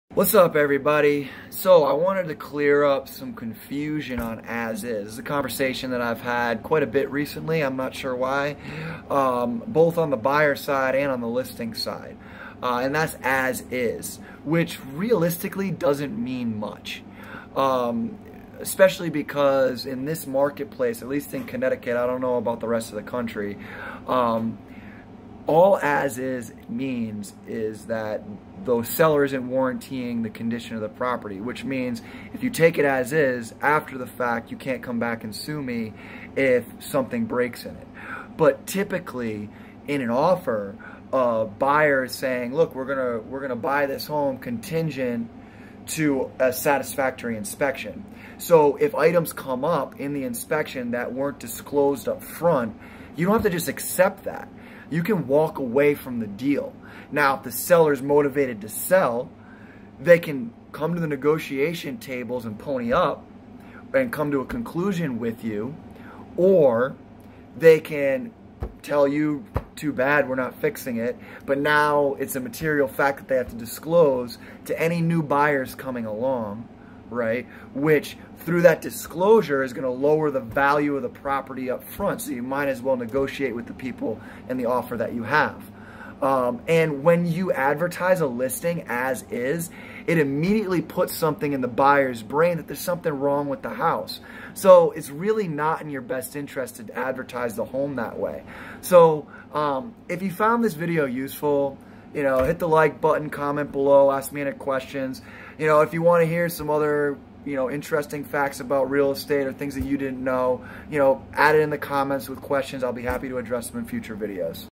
"As is" means nothing in real estate
🏡"As is" doesn't really mean much in a Real Estate sale🤯
In this video: ✅I clear up its's true meaning and the pit falls in advertising a property that way ❓How do you as a buyer negotiate condition on that kind of property❓